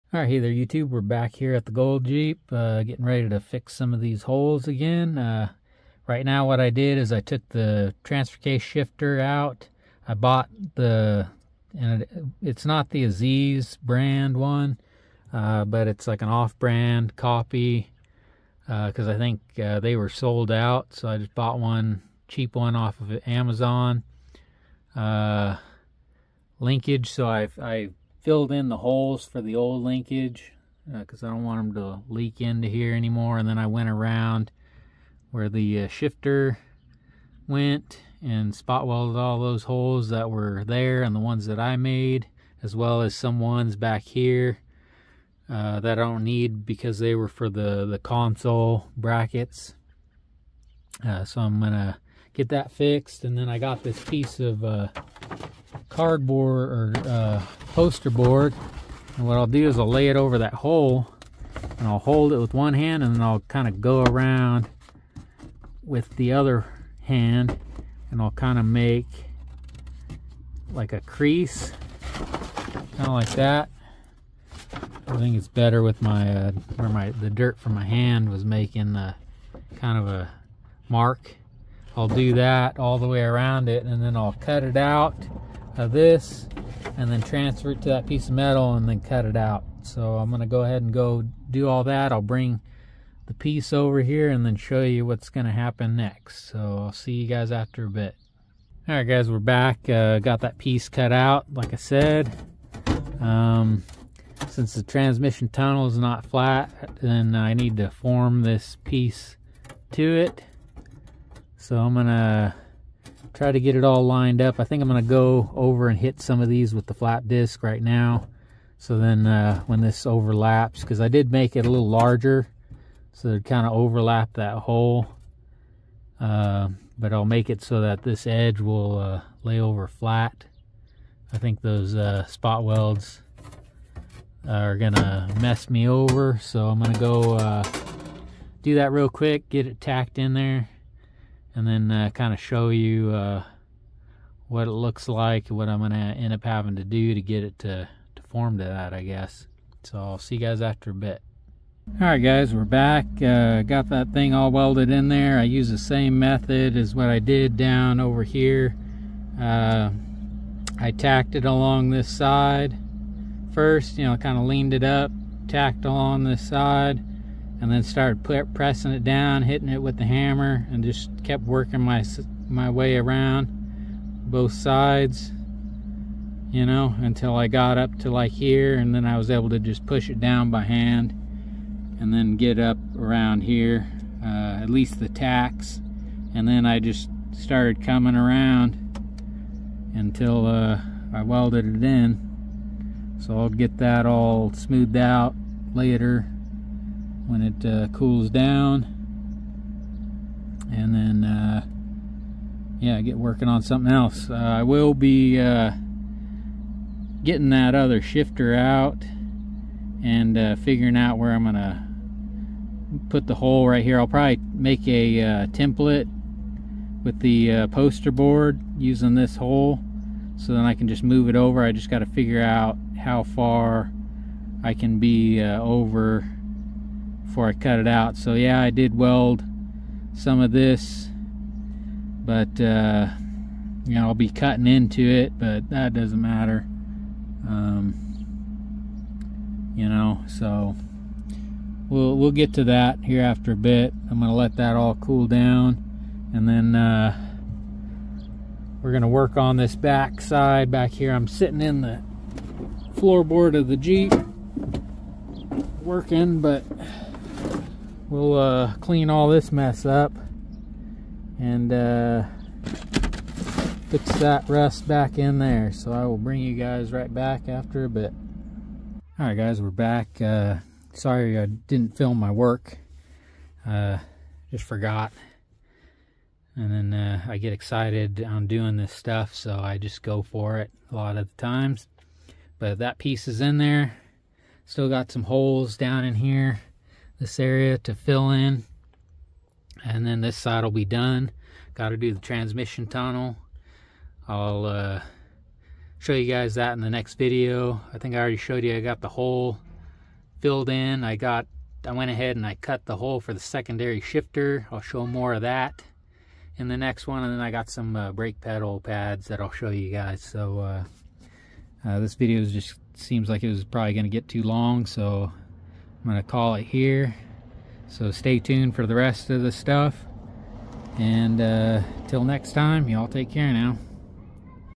More Rusty Floor Board Repair On The Gold Jeep ** Part 2**
In this video I continue to work on repairing the rusty Floorboards in the Gold Jeep. As far as the rust goes its pretty much done minus like 3 small holes. The Next Video I will cut and modify the transmission tunnel for the future Double and Transfercase, hopefully I will have my top secret shifter mods done so I can include that.